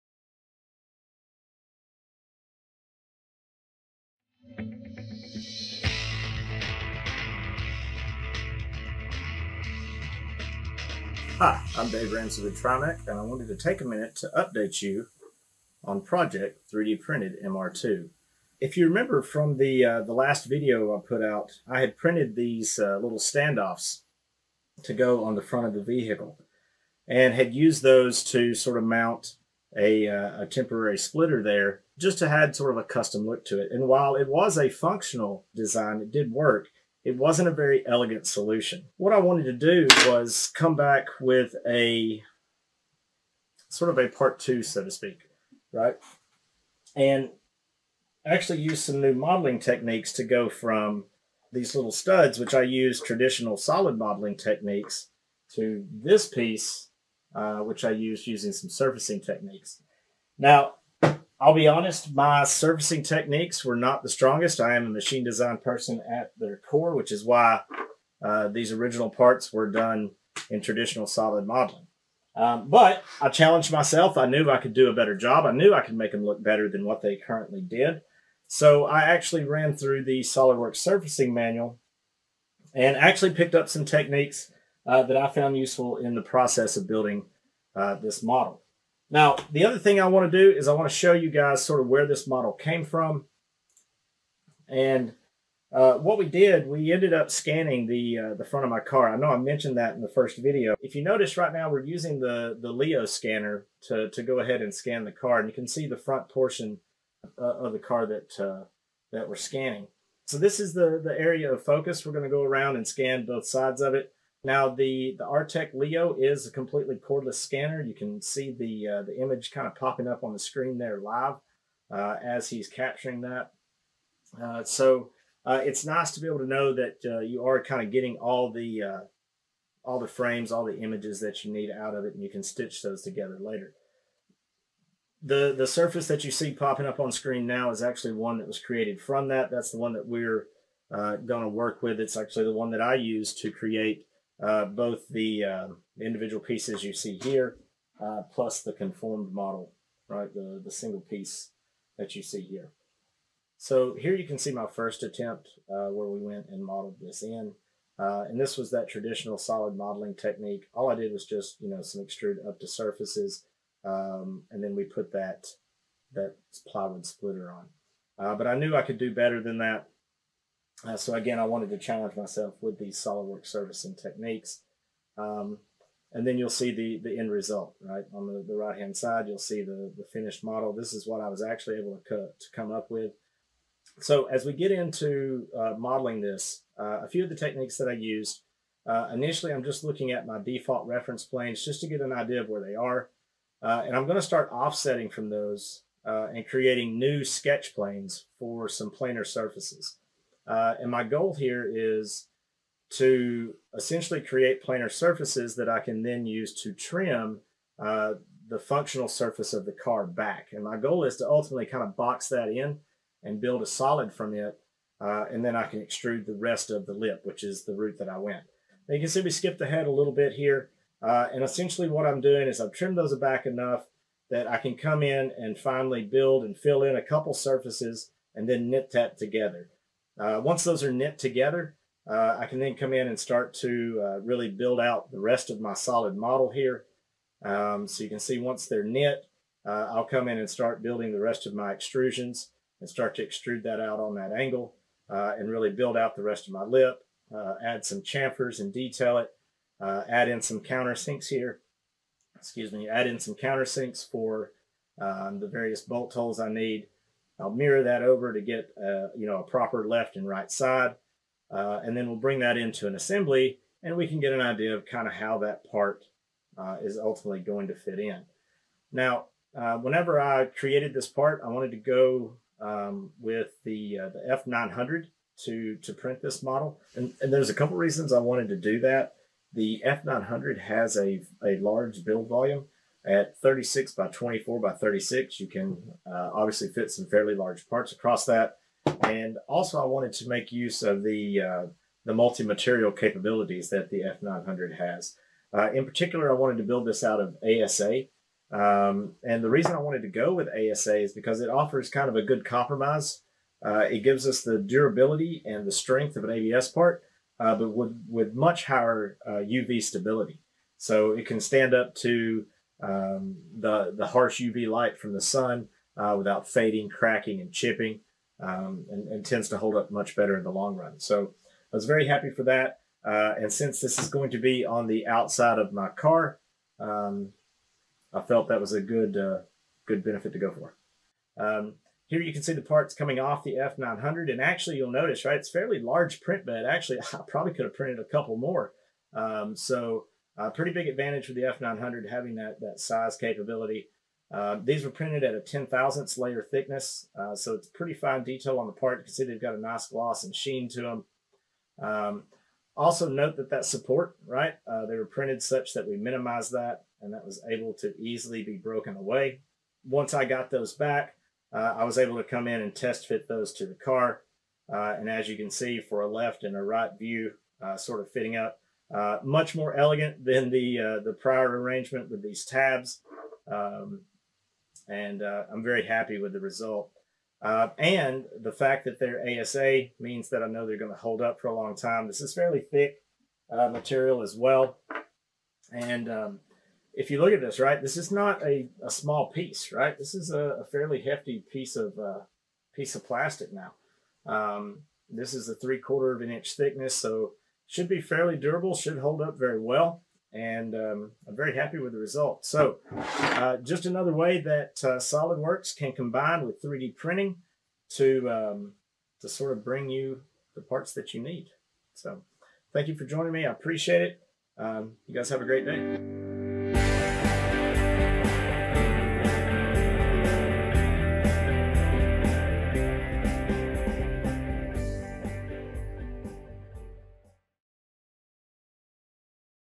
Creating an Automotive Wind Splitter Using 3D Scanning, SOLIDWORKS Surfacing and 3D Printing
For this video, I am scanning the front of my car so I can create a new wind splitter using SOLIDWORKS surfacing. I will then 3D print the wind splitter and mount it to my car. Let’s look at the steps I took to build a new version of my wind splitter.